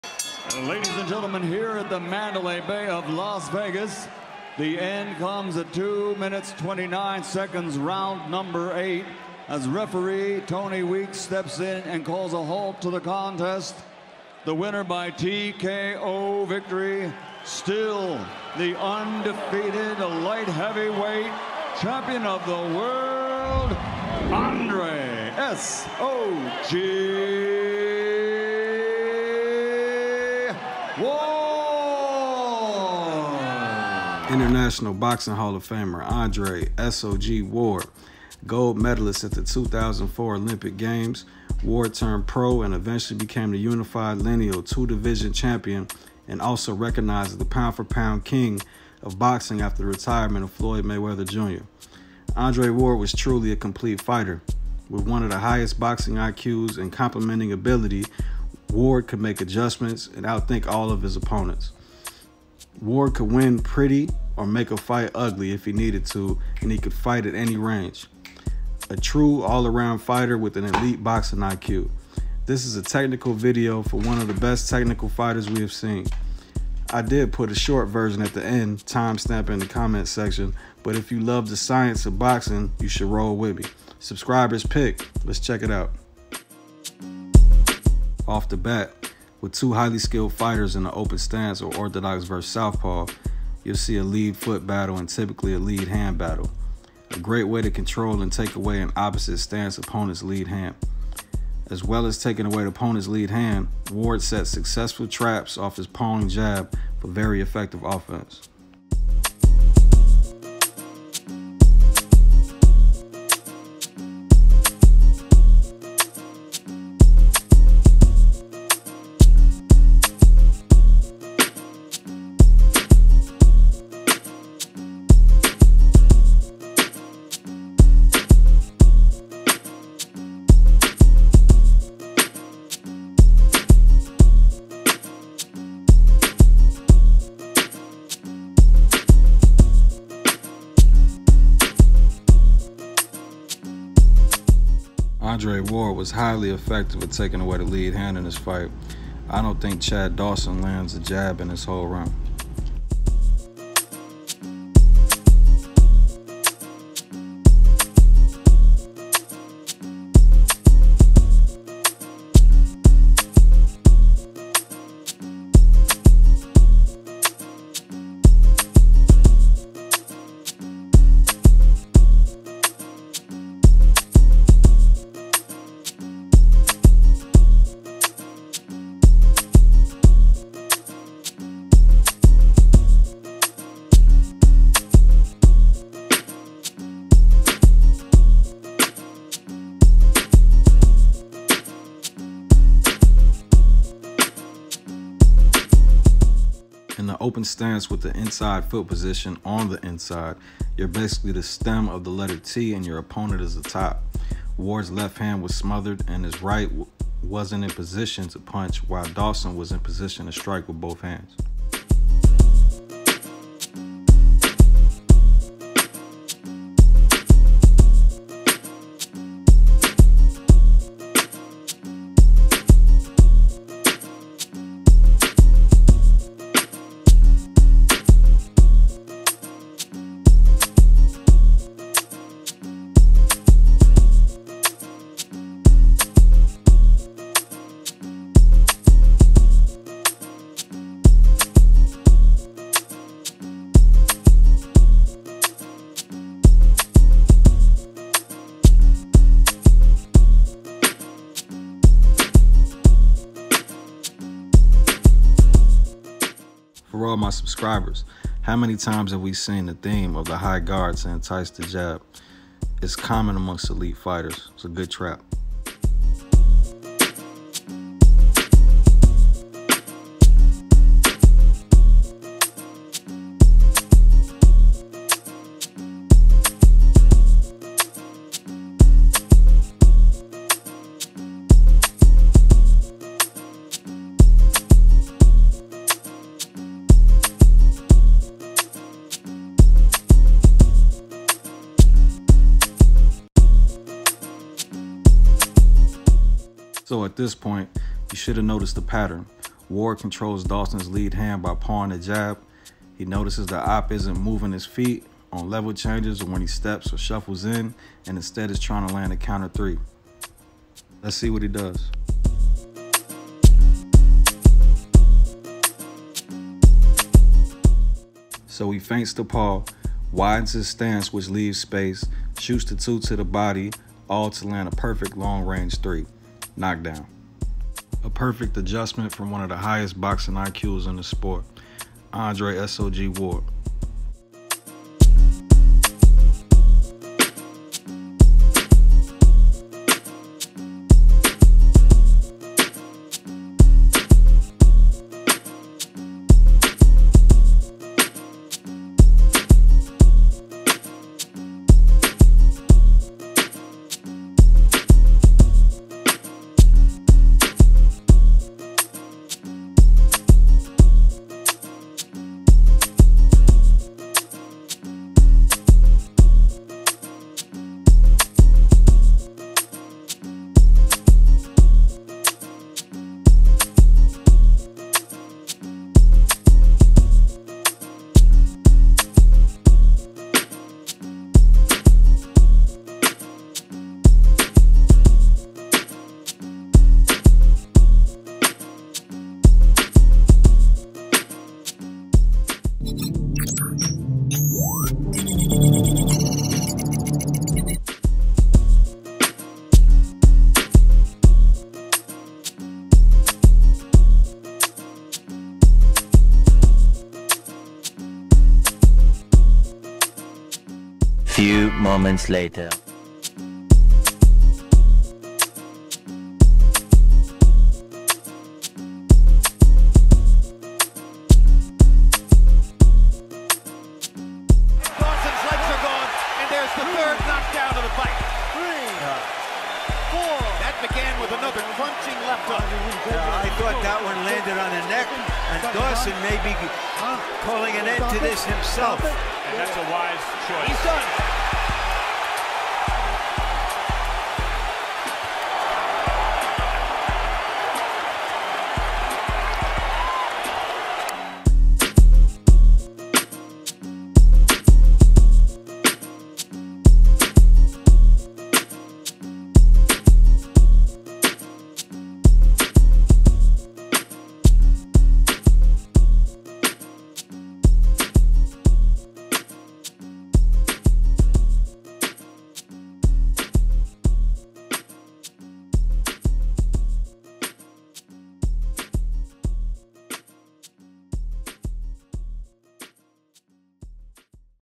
Andre “SOG” Ward: BOXING IQ - boxing breakdown
Andre Ward boxing IQ is a boxing film study focused on the 2 division Champions mental capacity and his ability to make adjustments on the fly and fight intelligently in a boxing ring.

Soundtrack by Rize of Upper Echelon

Every visual in this video was edited by boxing gyms solely.